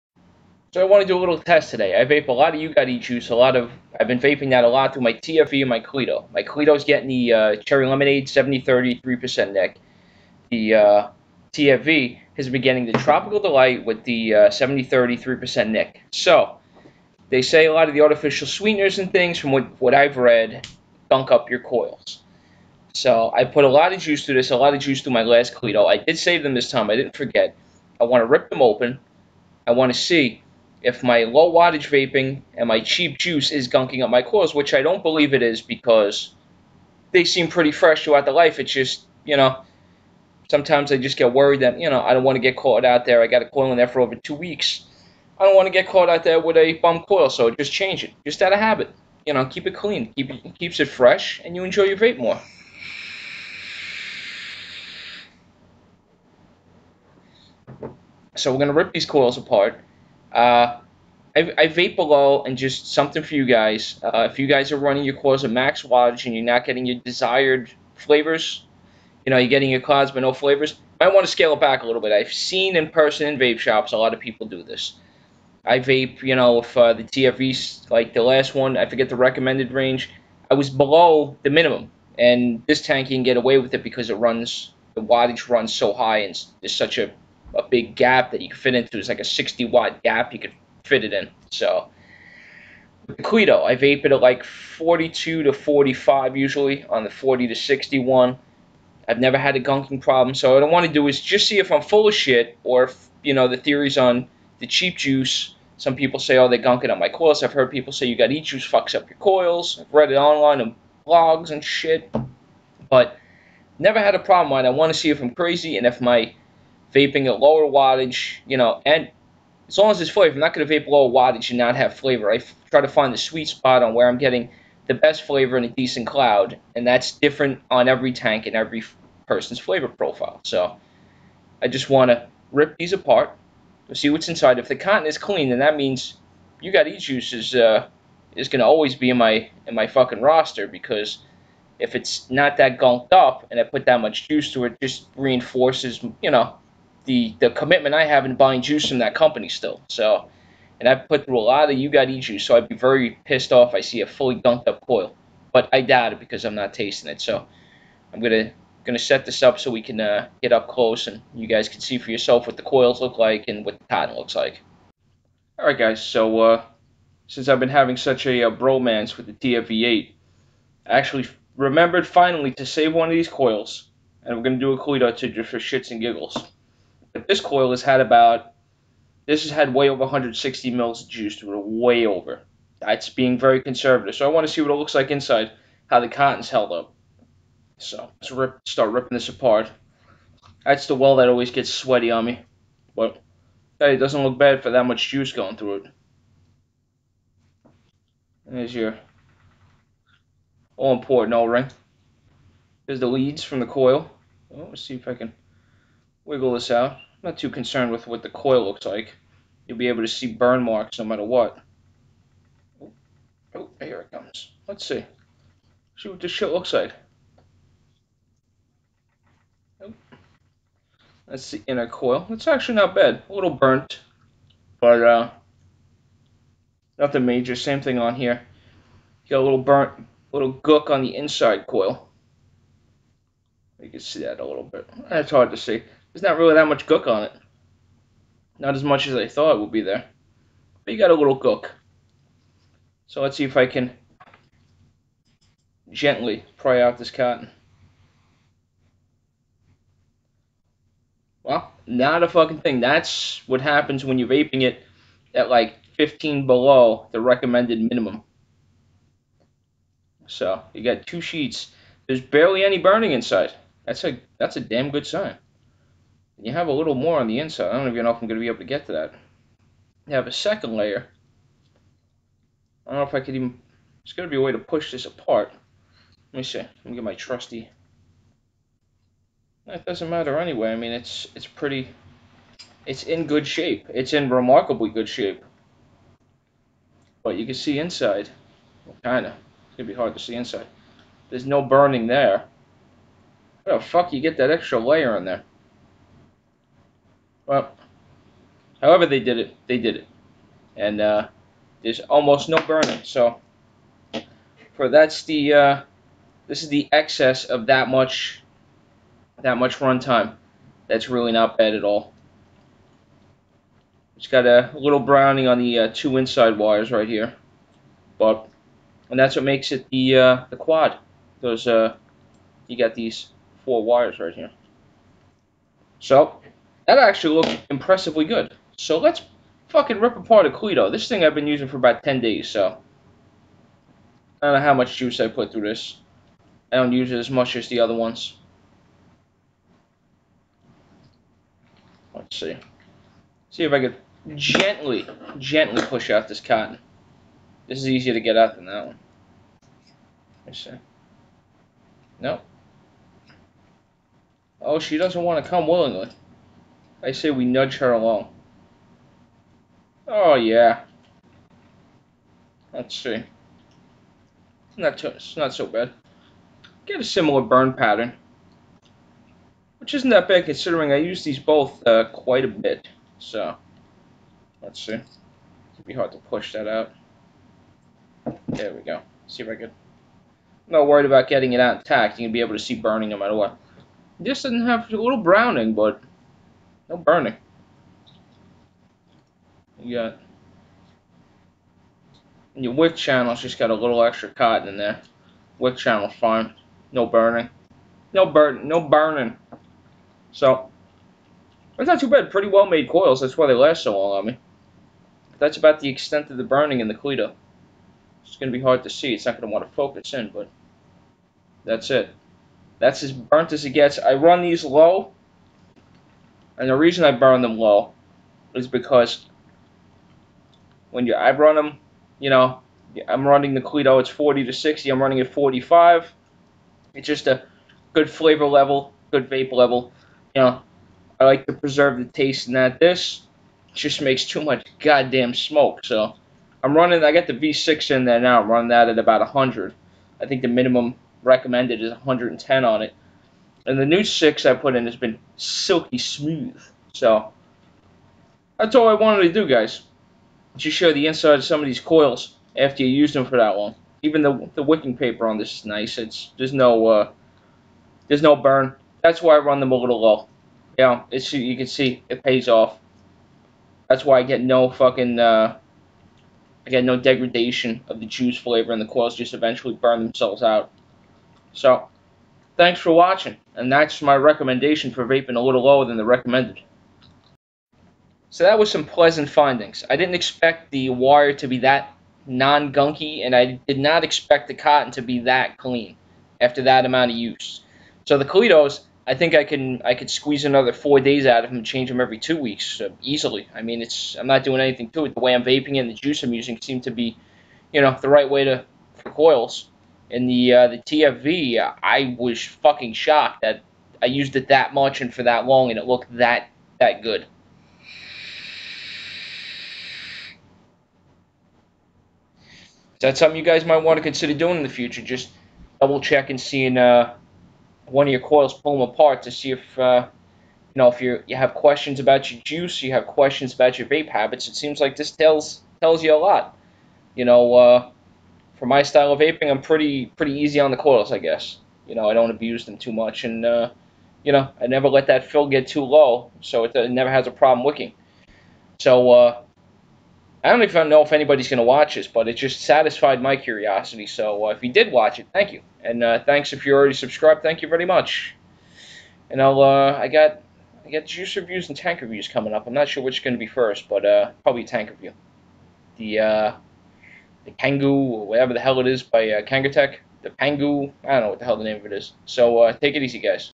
Coil Dissection-Does cheap -juice and chain vaping gunk up coils?
To satisfy my curiosity about my chain vaping You Got E-Juice on the TFV 8 and Cleito I break down two very used coils, enjoy.